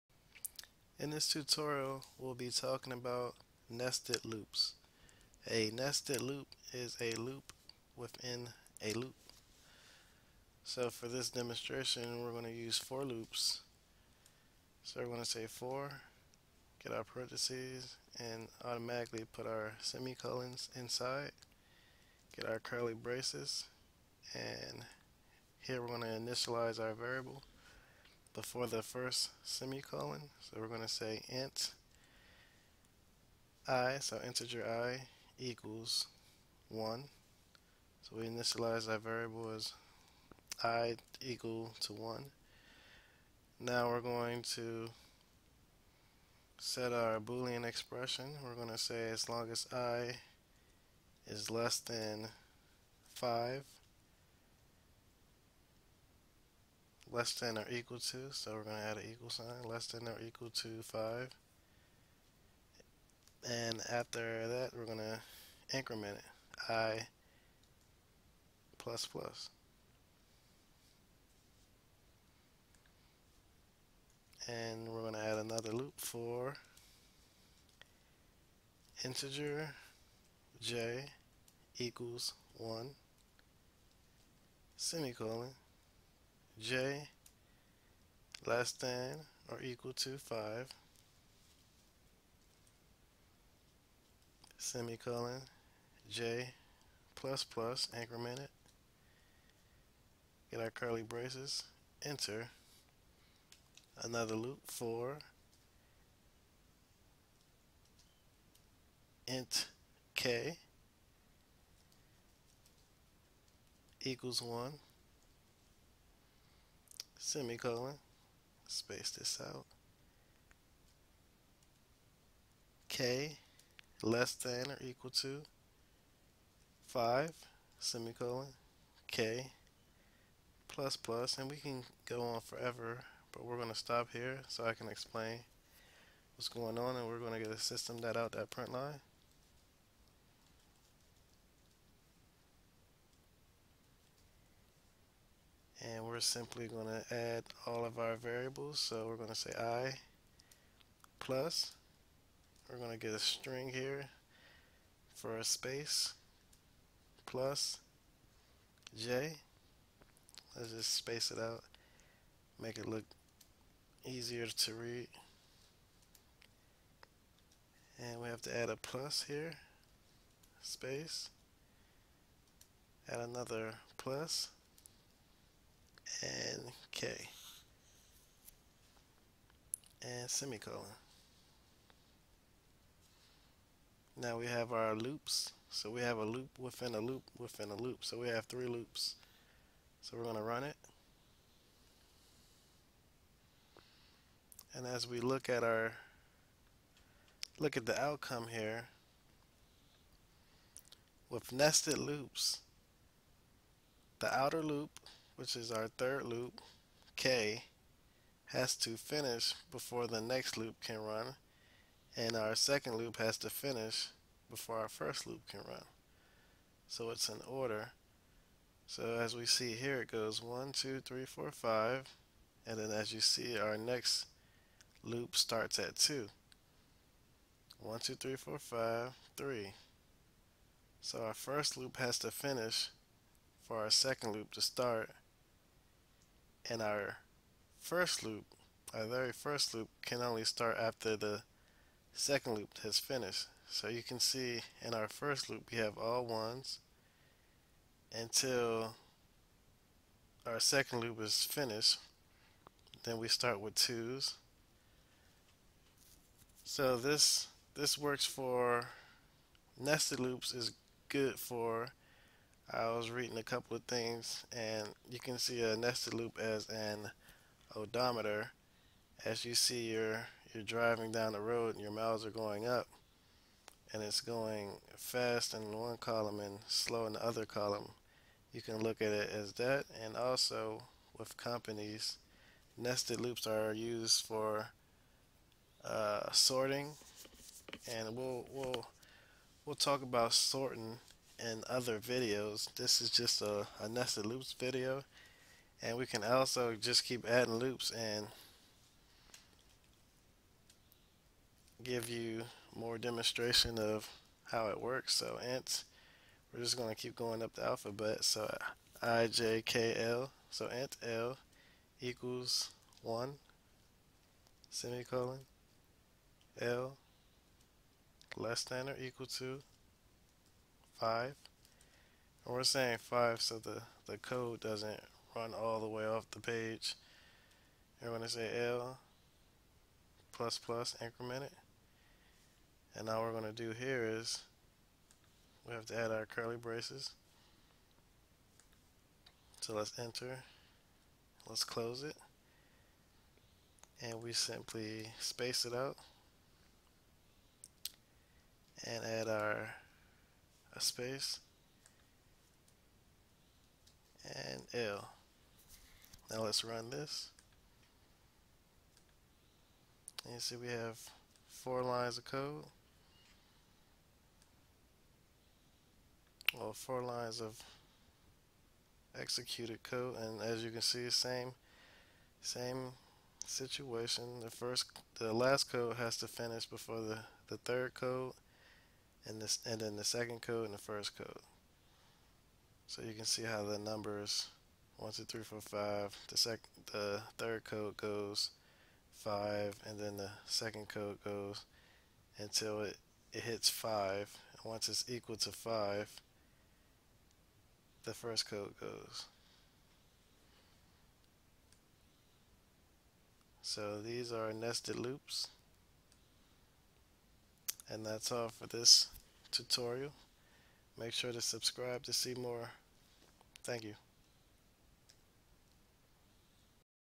Easy Java: Nested Loops
This tutorial is a demonstration of nested loops using the java programming language. Nested loops are great for sorting big data. Java beginner nested loops for those learning the coding language. In this tutorial, I used the Eclipse IDE. Computer Science students are welcome.